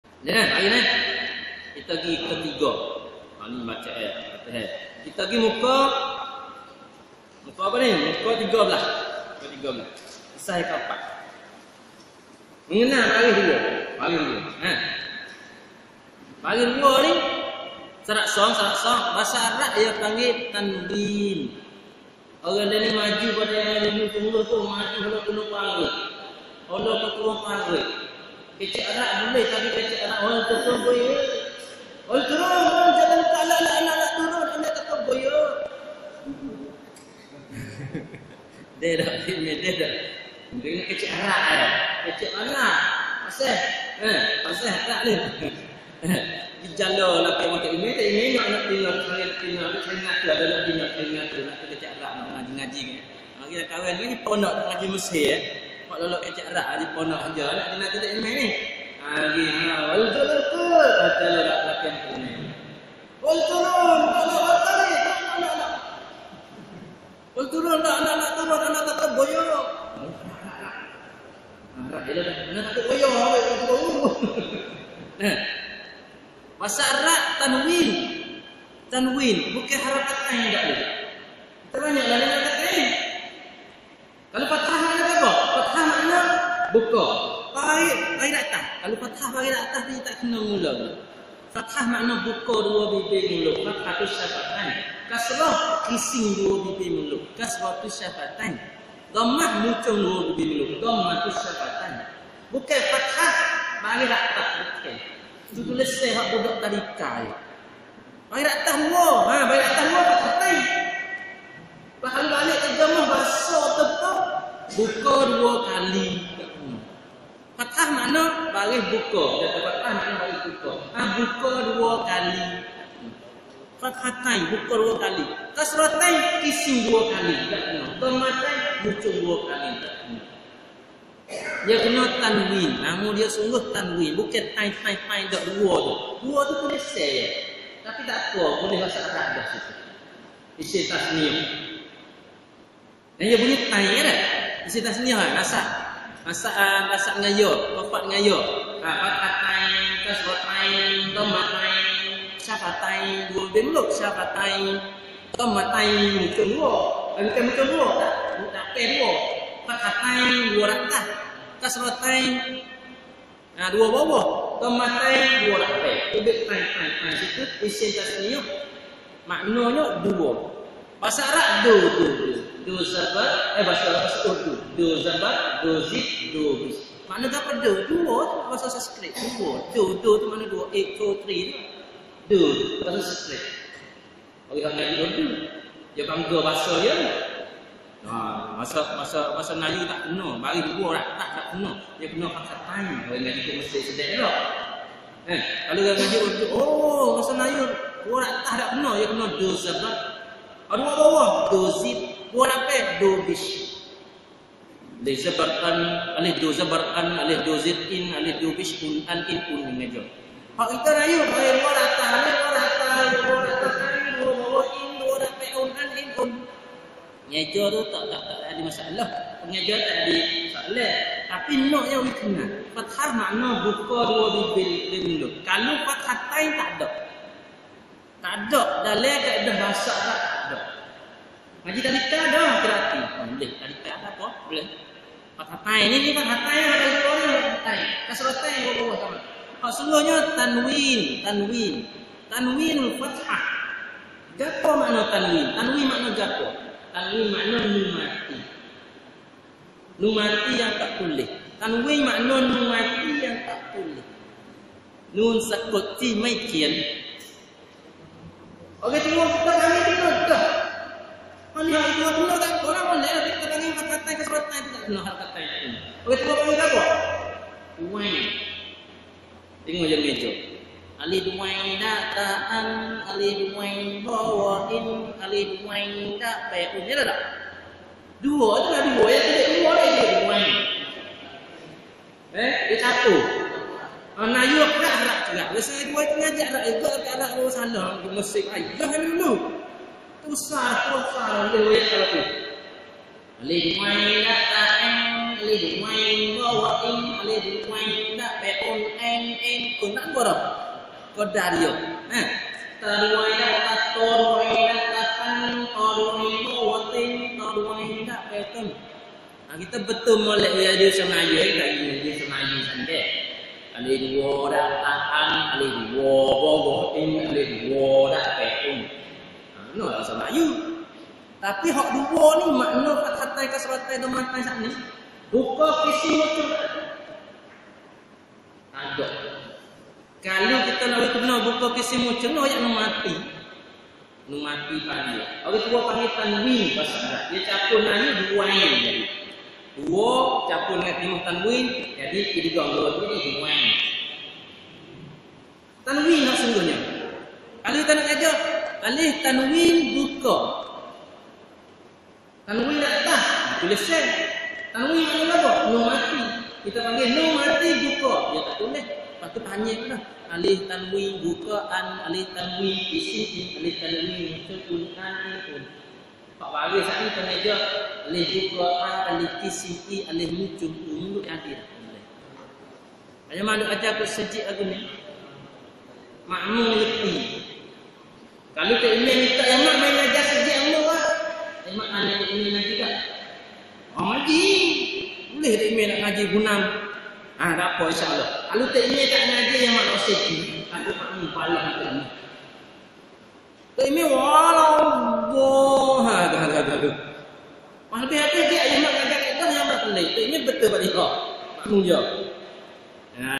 Asas Tajwid Ayah Mad Kulliyah imam Pattani (baris dua)วิทยาลัยอีมาม เรื่องสระสอง
majlis agama islam pattani